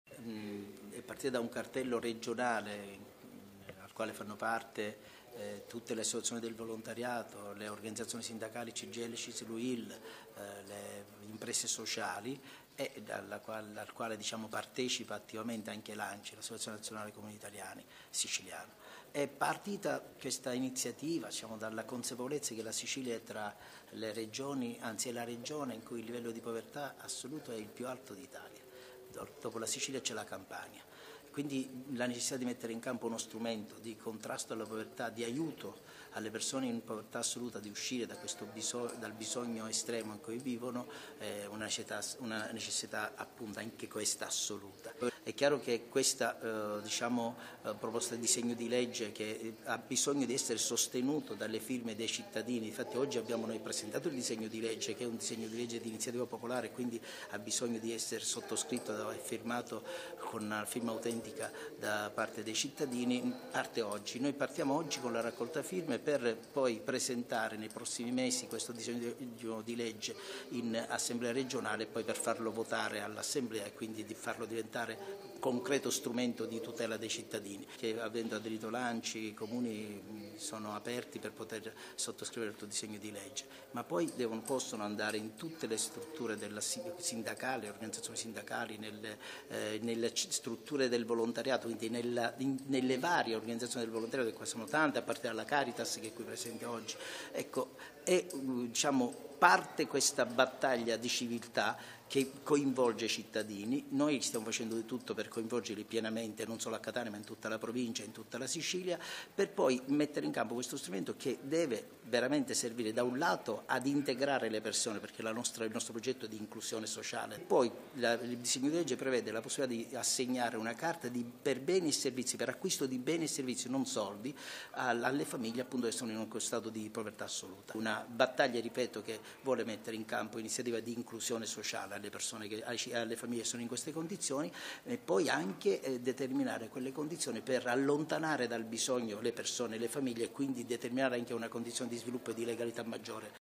Angelo Villari, assessore al Welfare del Comune di Catania
L'assessore al Welfare del Comune di Catania Angelo Villari sulla povertà assoluta